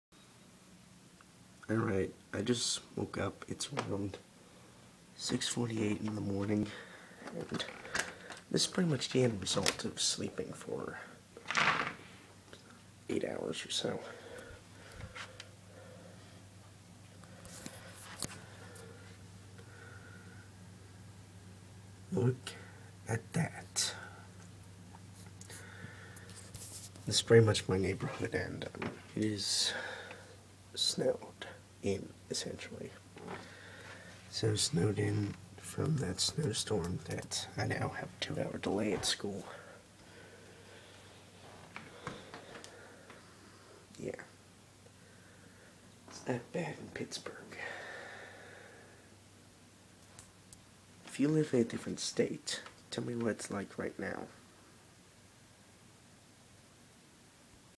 Waking up to Snow
I took a video with my Ipod Touch about how much it snowed overnight. And I know my face looks horrible! Enjoy